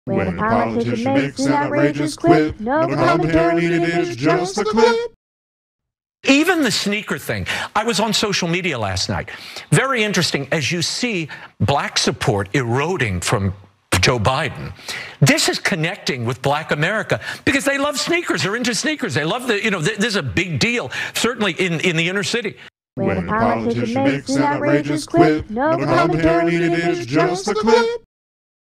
Just The Clip: Donald Trump Sneakers
A Conservative Pundit posits that Black voters will go for Trump because of his gold sneakers.

Remember to tune in to the podcast live, each Monday and Wednesday at 8PM Central here on YouTube.